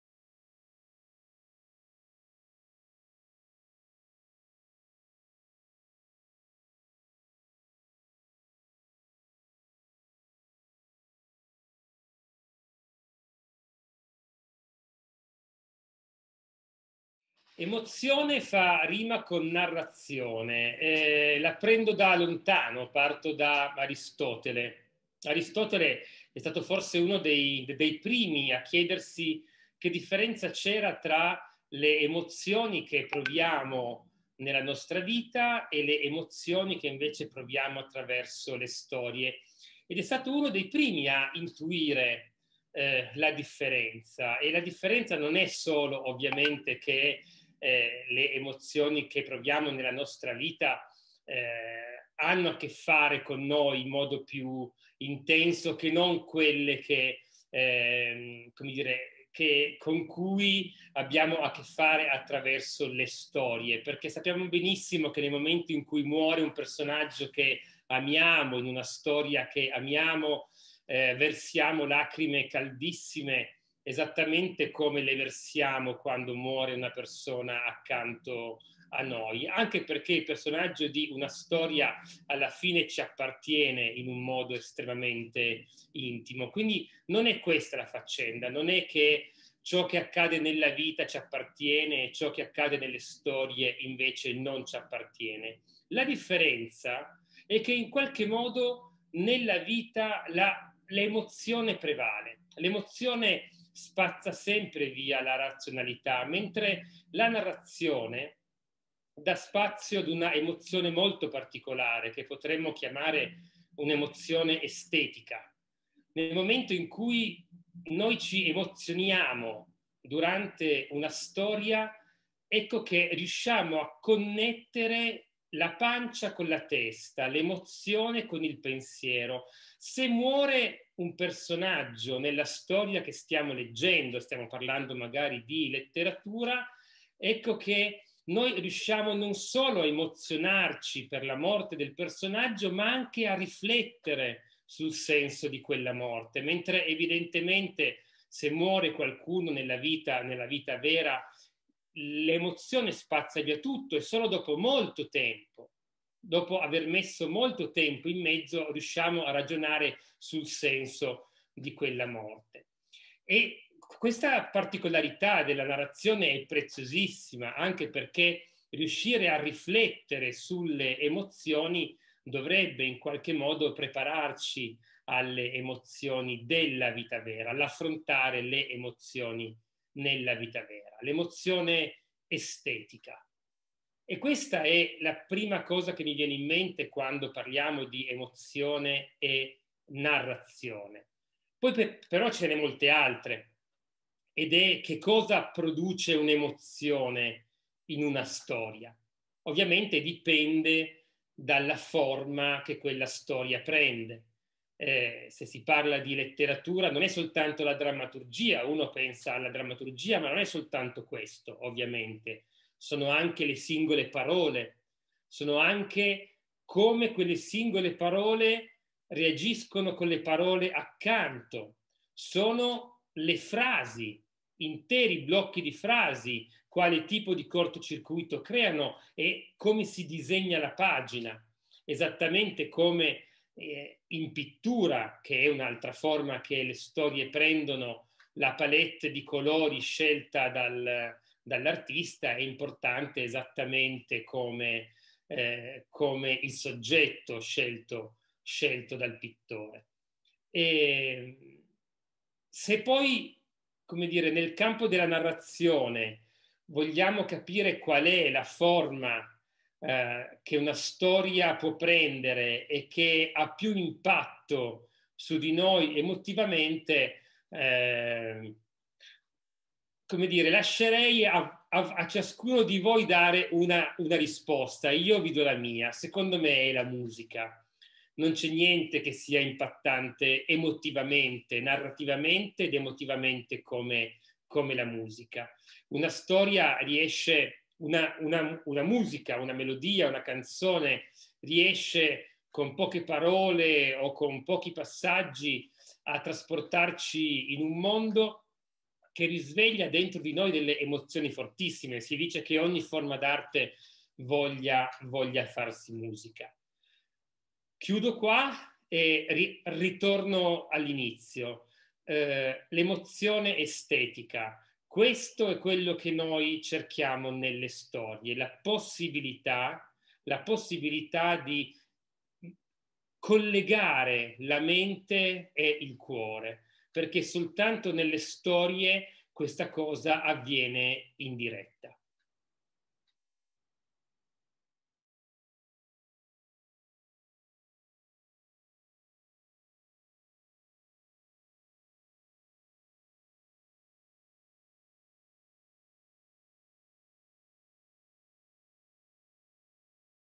DEPAL Fabio Geda - emotion rhymes with narration
Fabio Geda is a great novelist and an amazing writer. He shares with us how emotion and narration are linked, how they are connected and bonds at same time, bonds which rhymes.